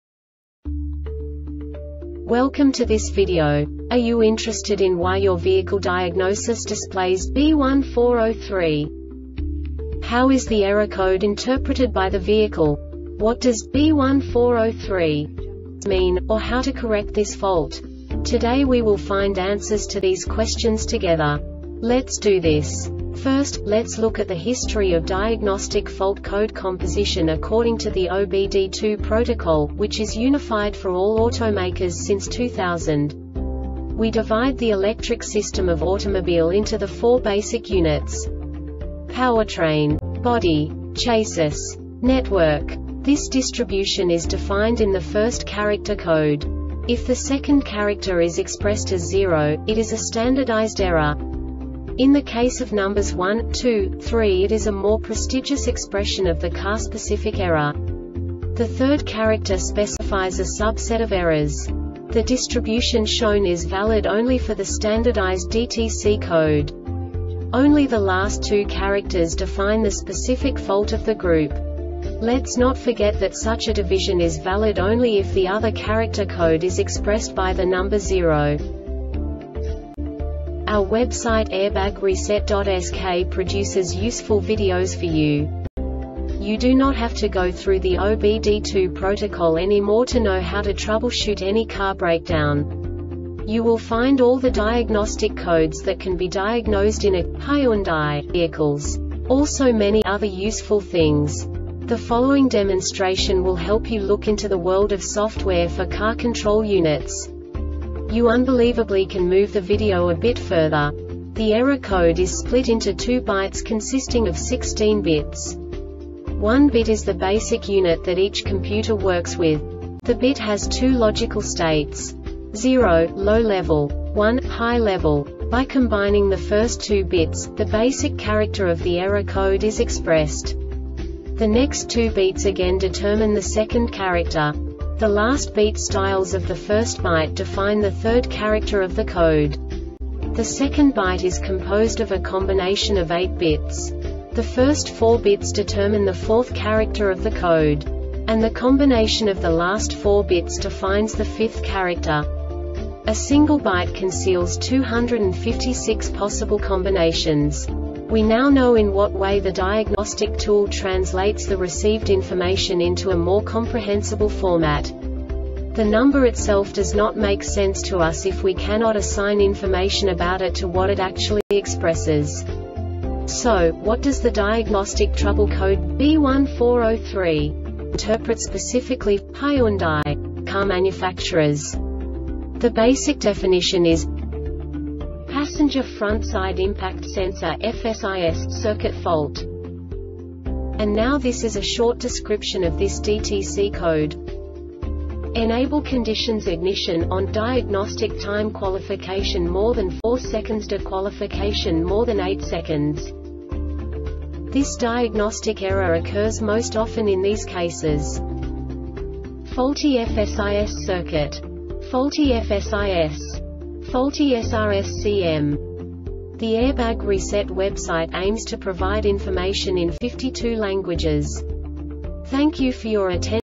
DTC Hyundai B1403 Short Explanation
Contents:
0:21 Basic DTC analysis according to OBD2 protocol standard.
1:48 Insight into programming
2:58 A diagnostically understandable description of the Diagnostic Trouble Code
3:05 Basic error definition

Make:
Hyundai
Code:
B1403
Definition:
Passenger Front Side Impact Sensor(FSIS) Circuit Fault
Description:
Enable Conditions Ignition "ON" Diagnostic Time Qualification More than 4 sec De-Qualification More than 8 sec
Cause:
Faulty FSIS circuit.
Faulty FSIS.
Faulty SRSCM.